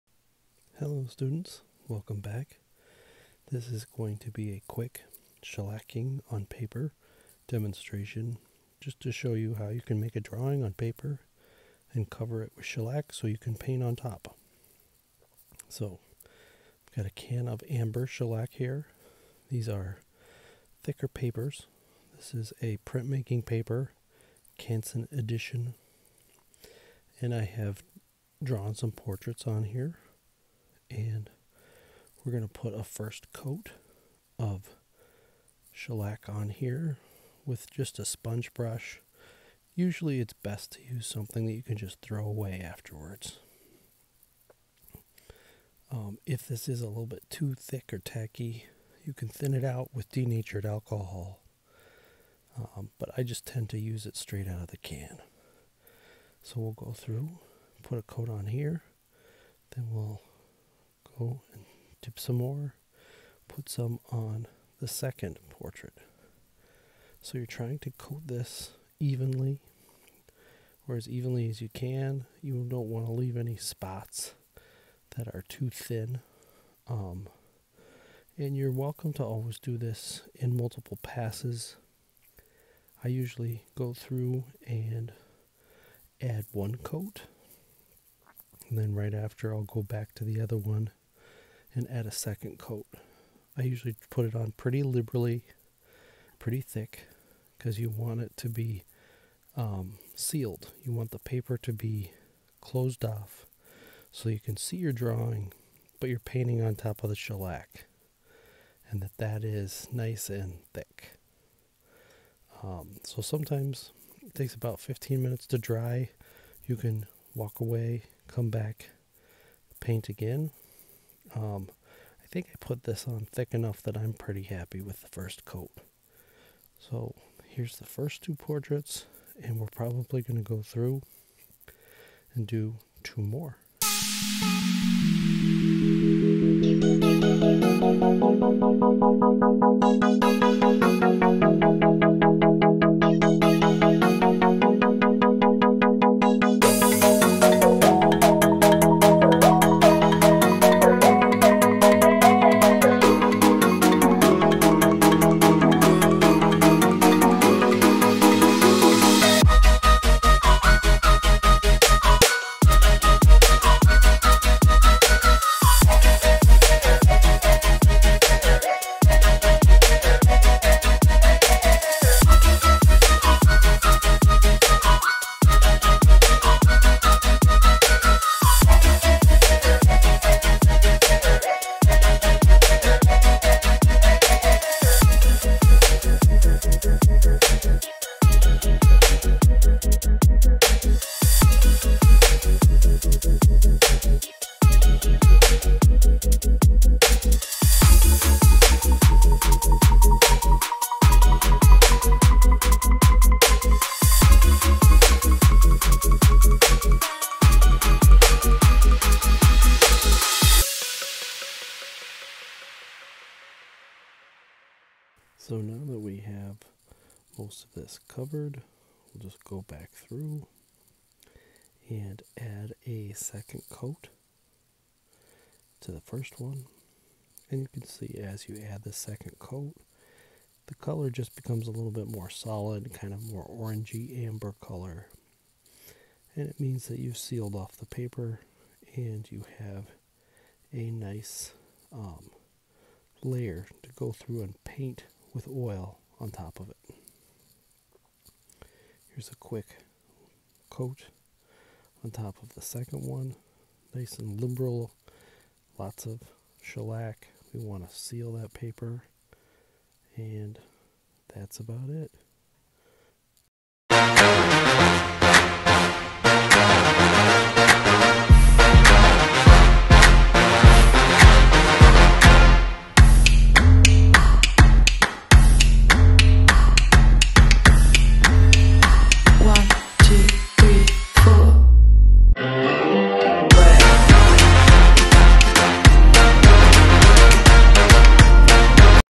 FA 218 Shellac Paper Demo
Adding a shellac coat to Canson Edition paper. Once dry you cannake  oil paint study on top.